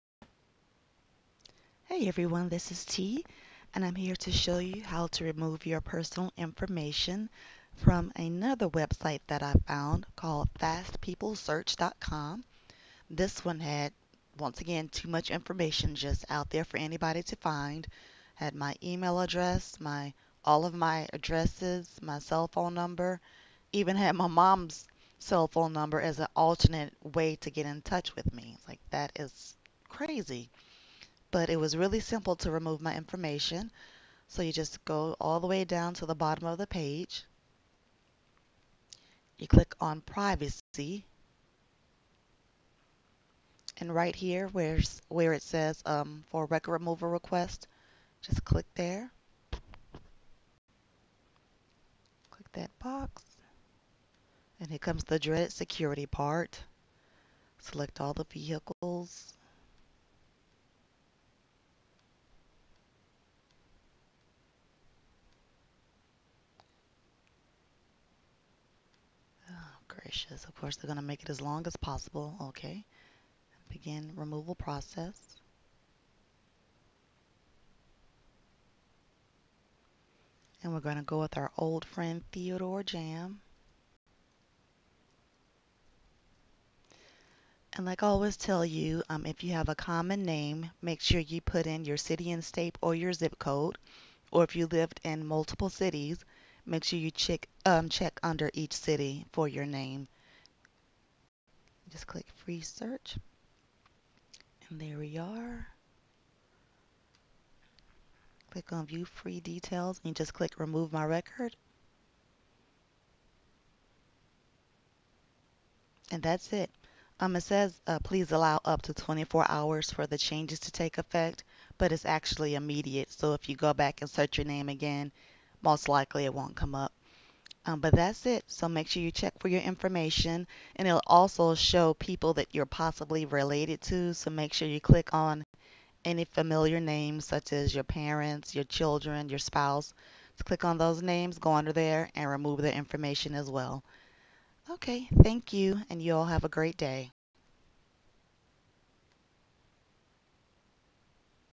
Remove Your Personal Information From FastPeopleSearch.com In 5 Minutes
This website had every piece of personal information on me and my family for free. All I had to do was type in my name and all of my information appeared within seconds.

Quickly and easily remove your personal information, and the information of your family, from FastPeopleSearch.com